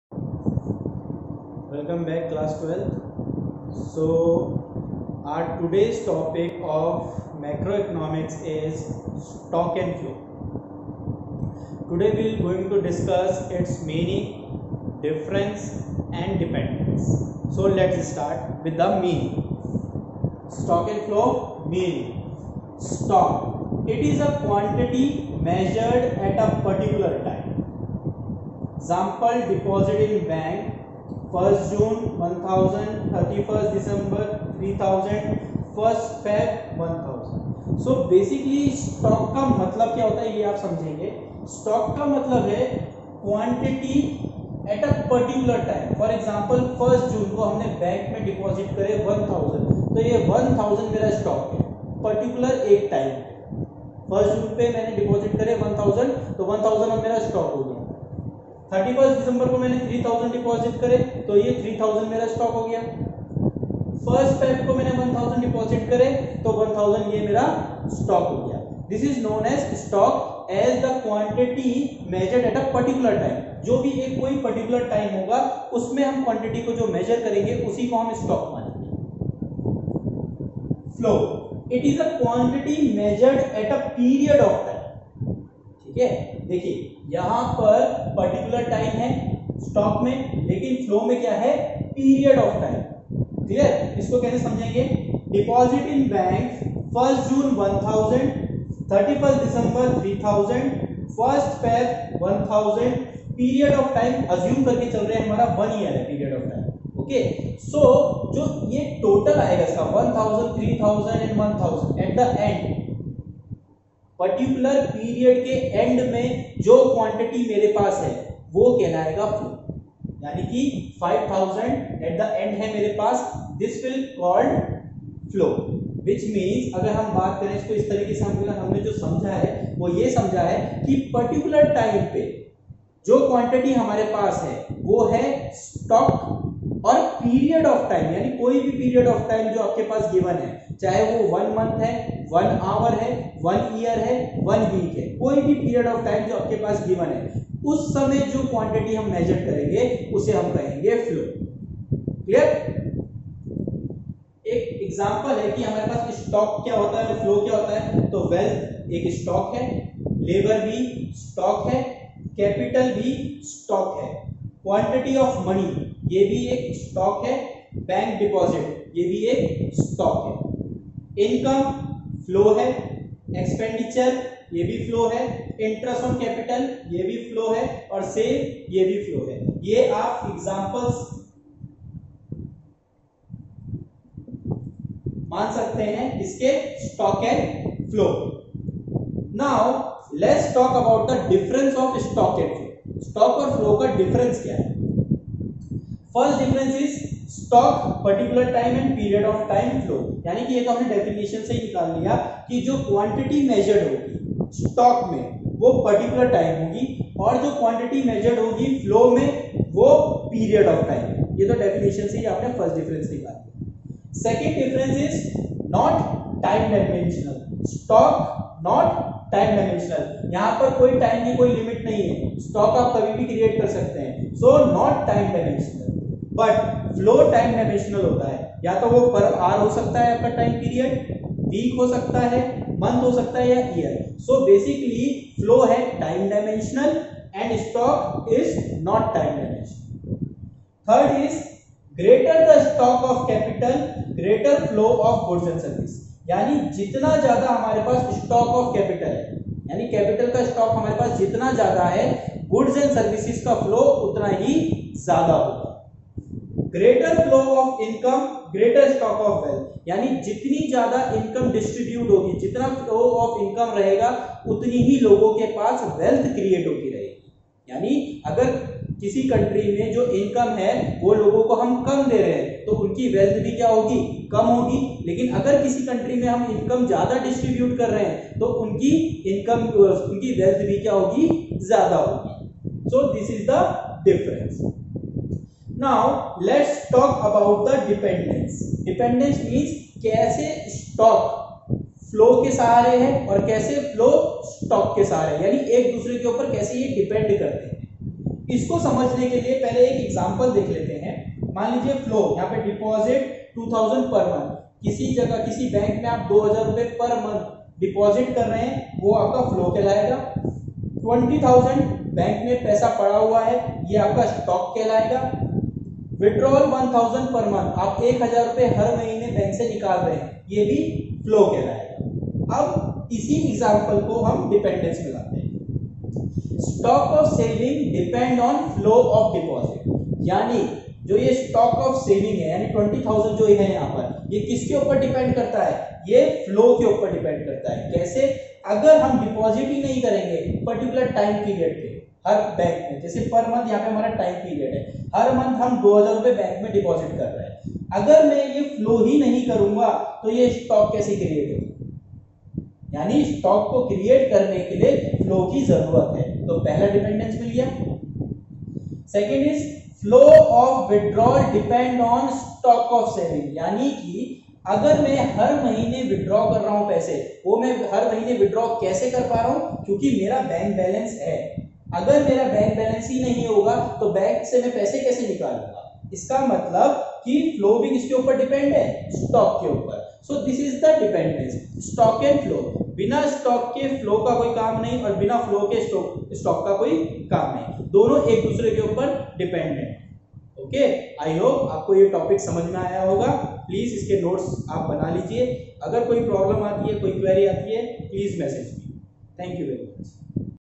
concept of stock and flow/difference between stock and flow commerce classes by rachit rawat
concept of stock and flow /money flow real flow diffrence between  stock and flow/ dependence of stock and flow
commerce classes by rachit rawat
commerceclassesbyrachitrawat